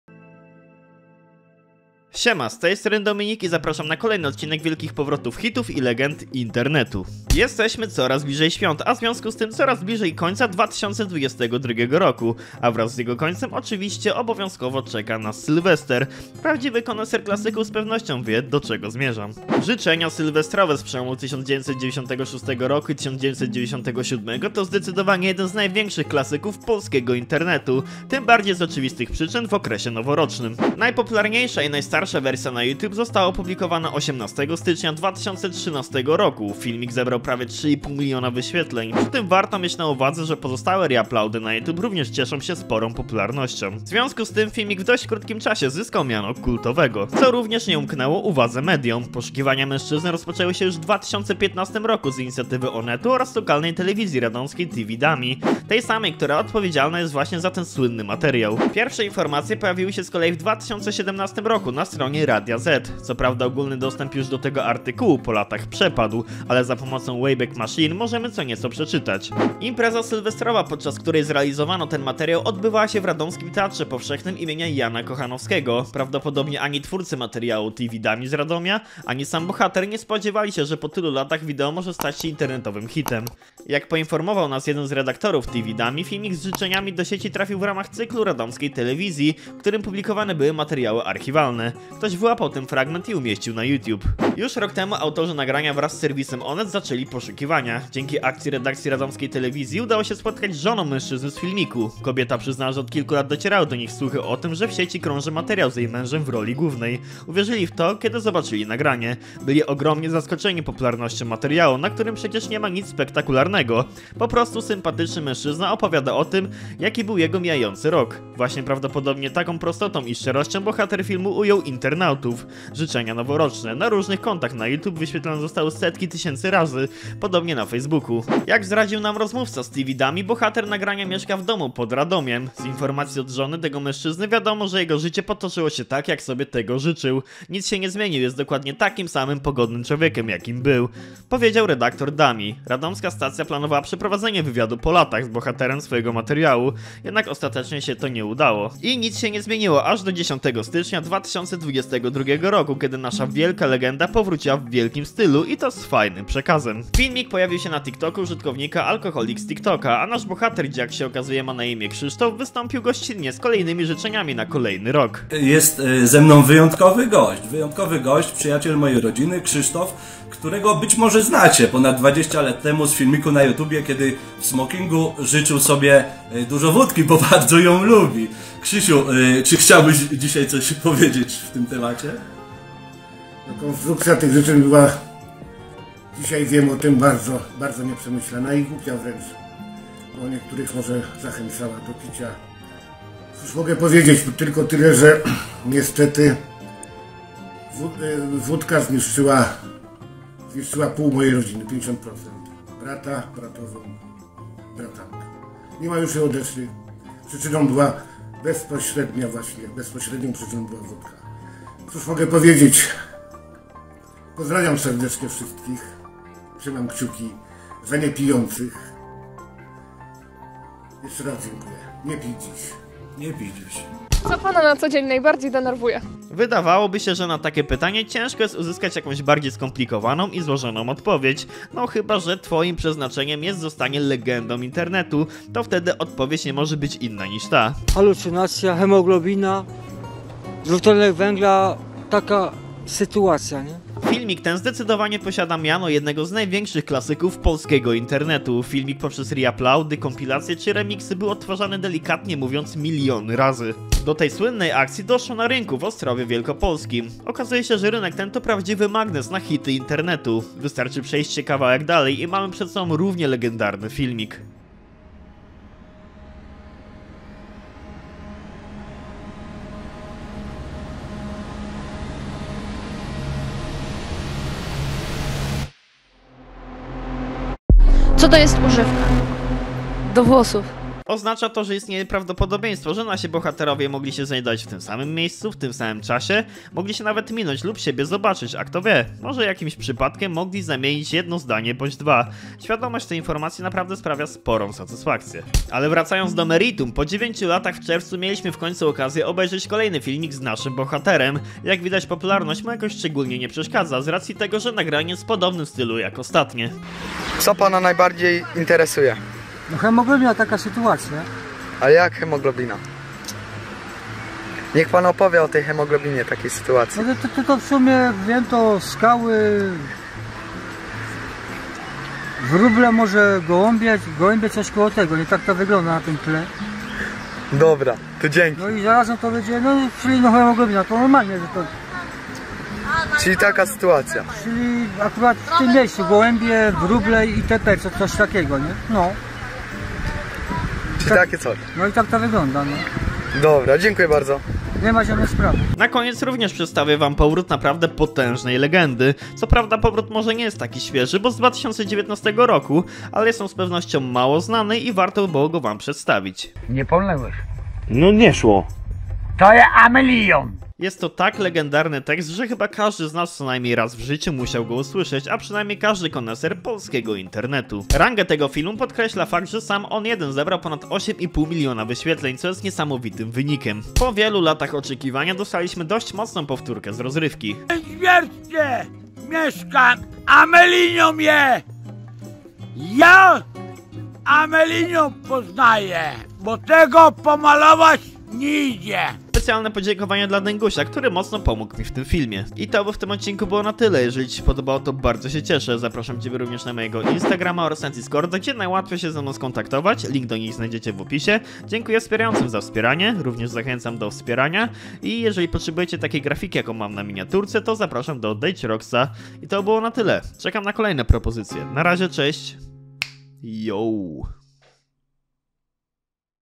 LEGENDY WRACAJĄ DO INTERNETU! (ŻYCZENIA NOWOROCZNE 96/97,HEMOGLOBINA TAKA SYTUACJA,TO JE AMELINIUM)
W dzisiejszym wyjątkowym odcinku omawiamy powrotu potężnych legend internetu 😎. Jeżeli chcielibyście odcinek na inny temat to śmiało zachęcam do komentowania jeżeli natrafi się temat najlepszy możliwie do zrealizowania.